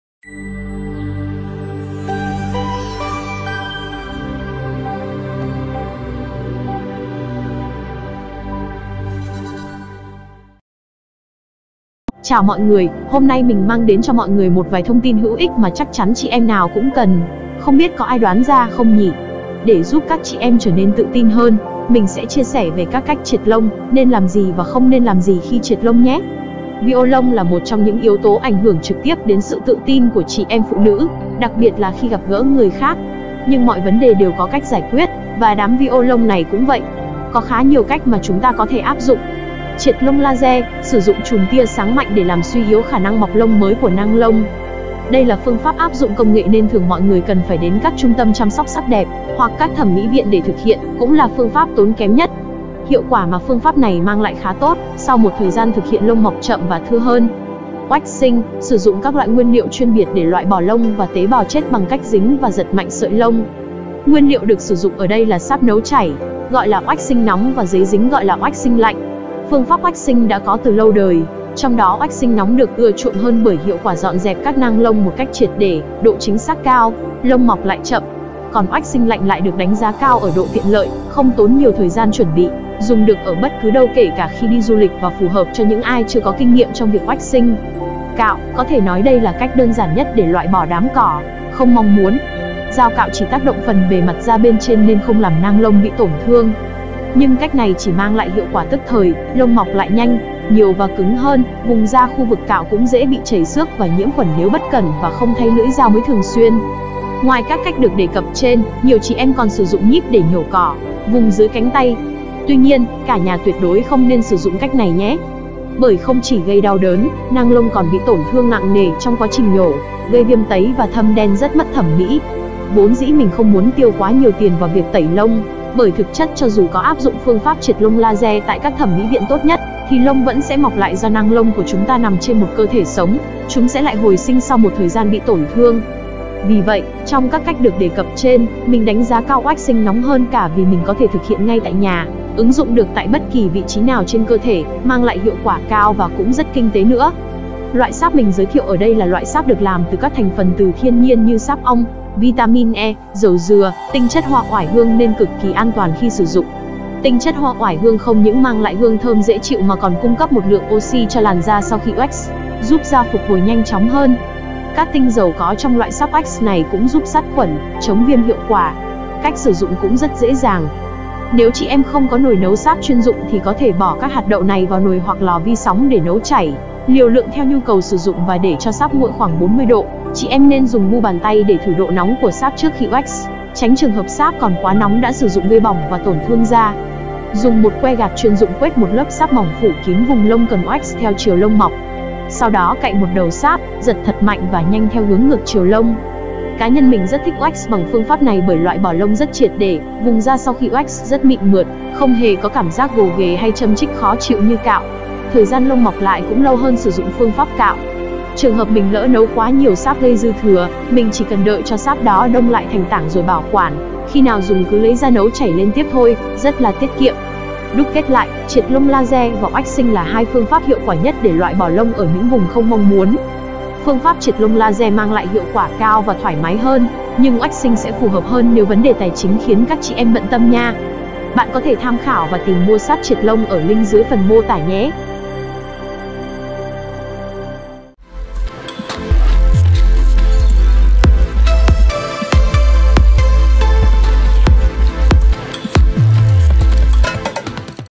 Bản Tin Xu Hướng Cộng | Tập 45 Full: Triệt lông ngay tại nhà một cách dễ dàng – Bạn đã thử chưa?
Đây là bản tin hàng ngày của Xu hướng +, bản tin sẽ mang đến cho mọi người những thông tin bổ ích về làm đẹp, thời trang và các tips.

Đừng quên cập nhật bản tin mới nhất của Xu Hướng + trên kênh Youtube và Facebook của Insiders.

00:00 Bản Tin Xu Hướng Cộng | Tập 45 Full: Triệt lông ngay tại nhà một cách dễ dàng – Bạn đã thử chưa?
00:40 Phương Pháp Triệt Lông Laser
00:58 Phương Pháp Waxing
01:28 Phương Pháp Cạo
02:03 Hard Wax Beans
02:49 Cách sử dụng